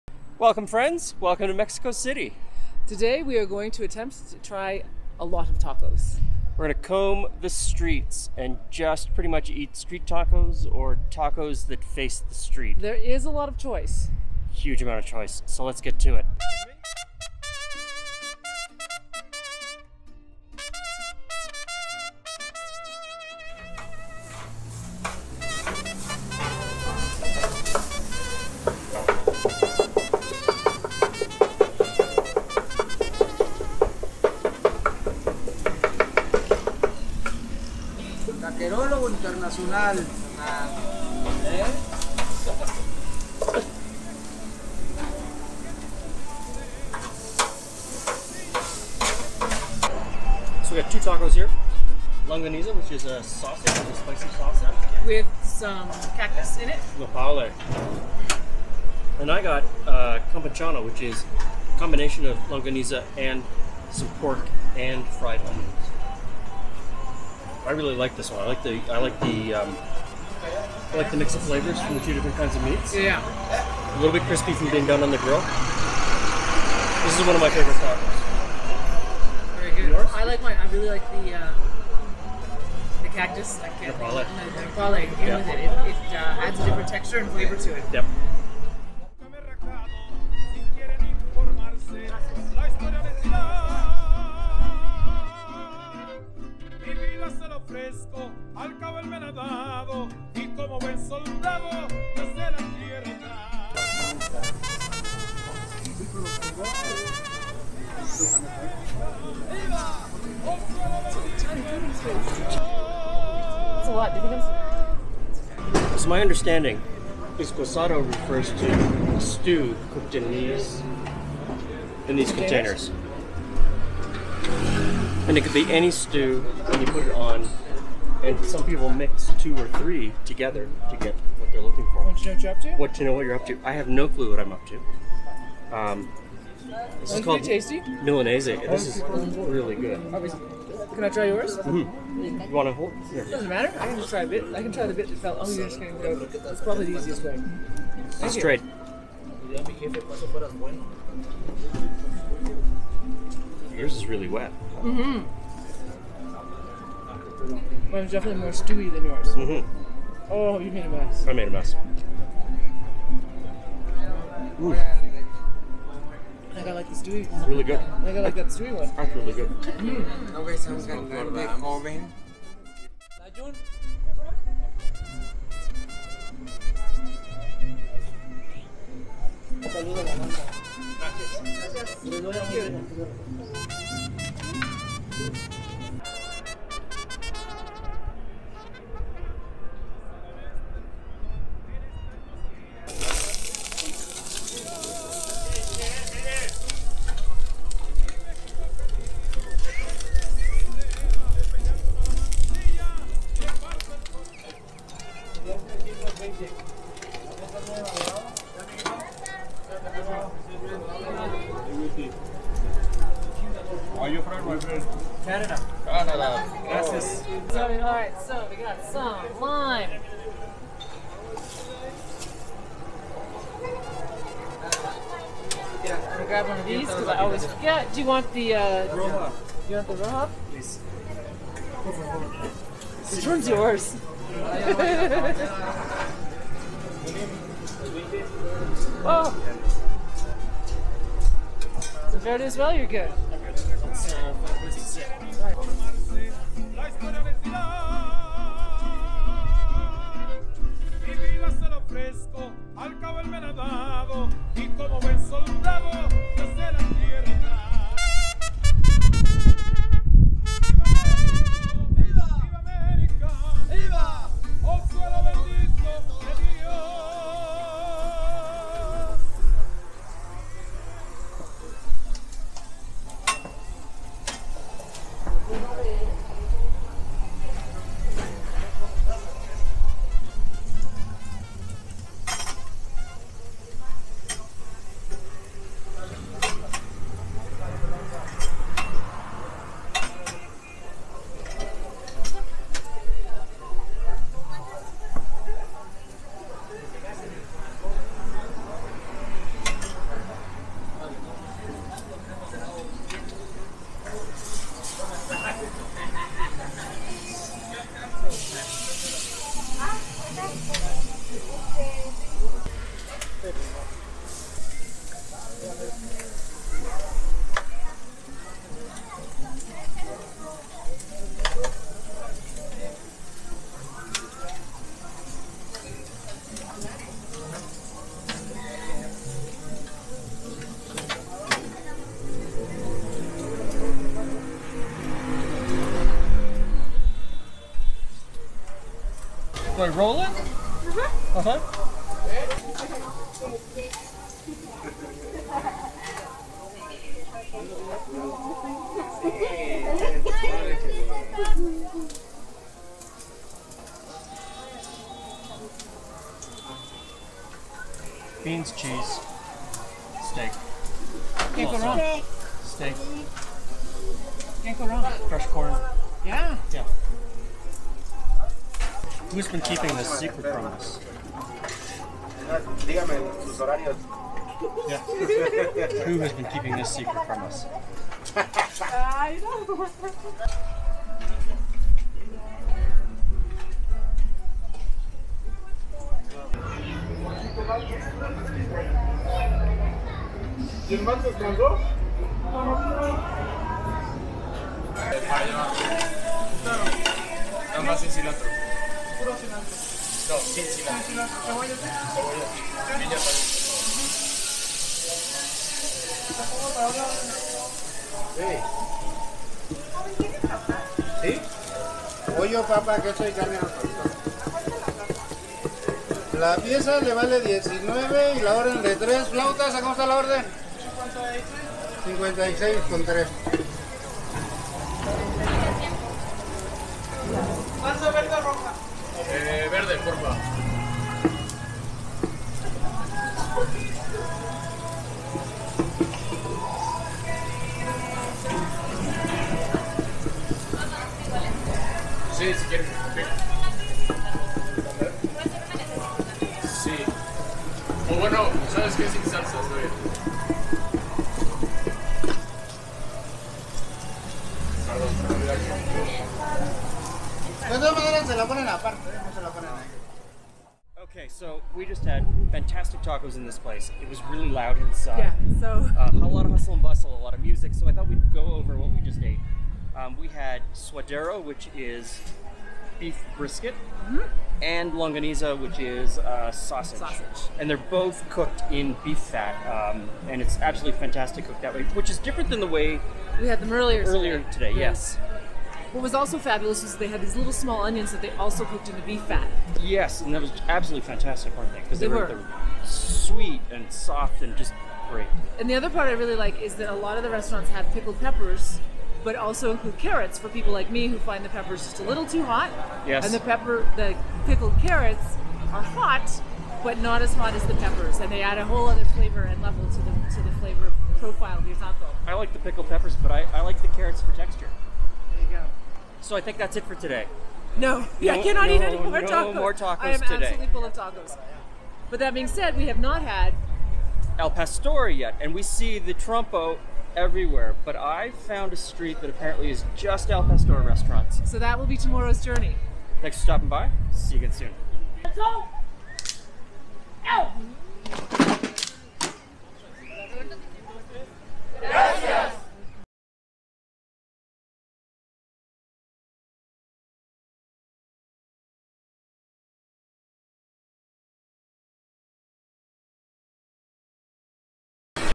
What We Ate In A Day - MEXICO City Street TACOS
What We Ate In A Day! Eating Street Food Tacos In Mexico City - this is a pretty straight forward street food vlog where we just eat Mexico City Street Food, specifically Mexican street food tacos. Are these the best tacos in Mexico City? Maybe, maybe not. But it is a street food compilation travel vlog taco tour of Mexico City food travel.

Le Gourmet TV Is Now - Glen & Friends Cooking!